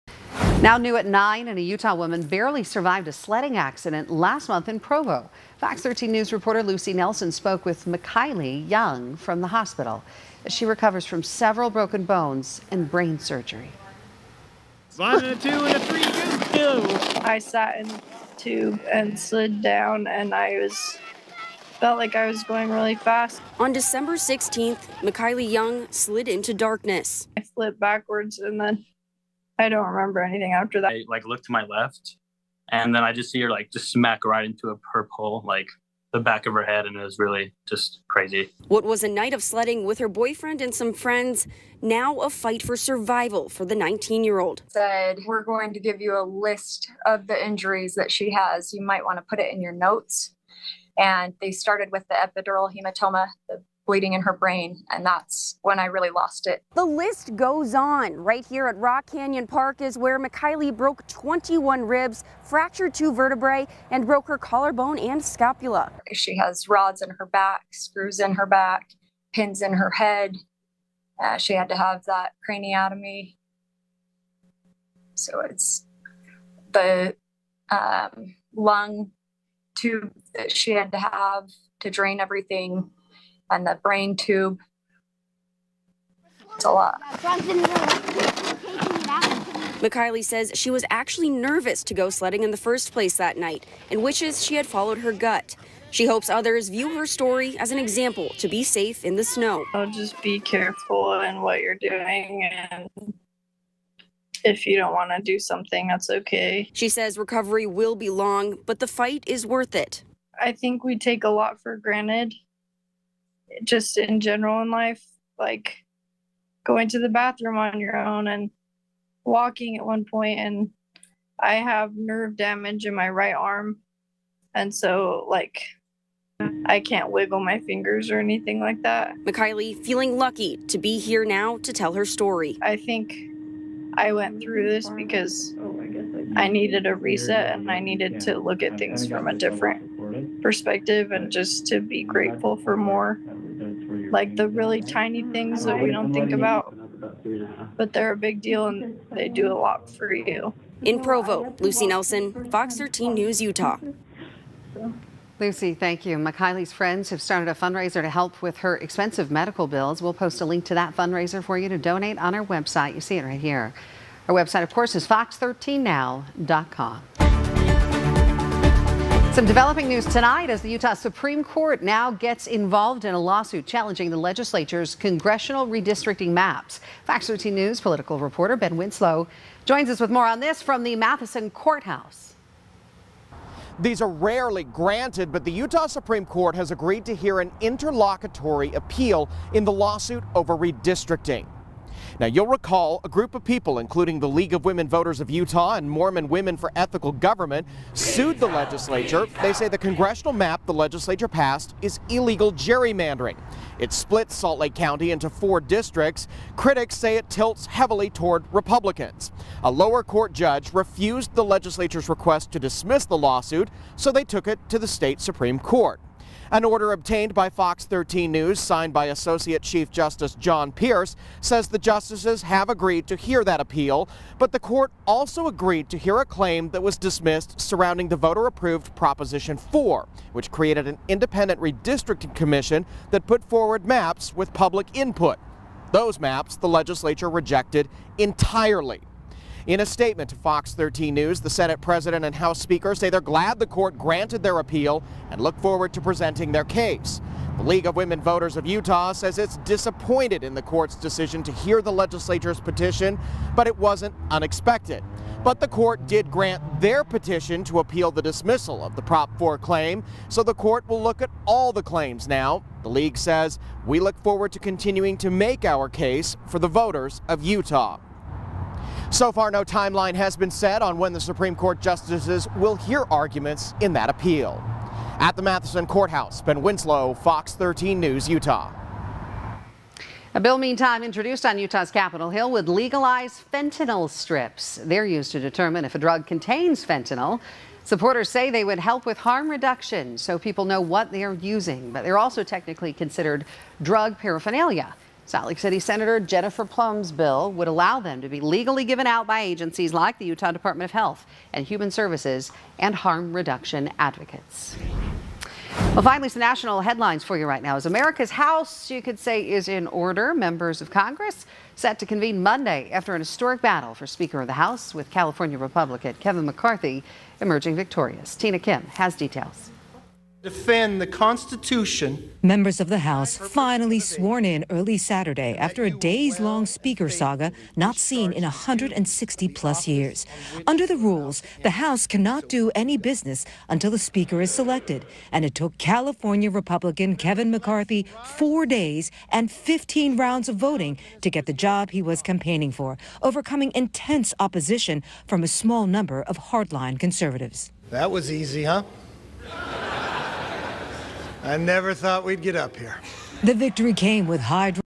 Utahn Recovering From Severe Sledding Accident
A Provo woman is still recovering from severe trauma after a sledding accident that left her with several broken bones and the need for brain surgery to recover from her head injuries.

Mckyliee Young,19, was tubing on December 16 when she says “I sat in the tube and slid down and I was I felt like I was going really fast . . . I flipped backwards and then I don't remember anything after that."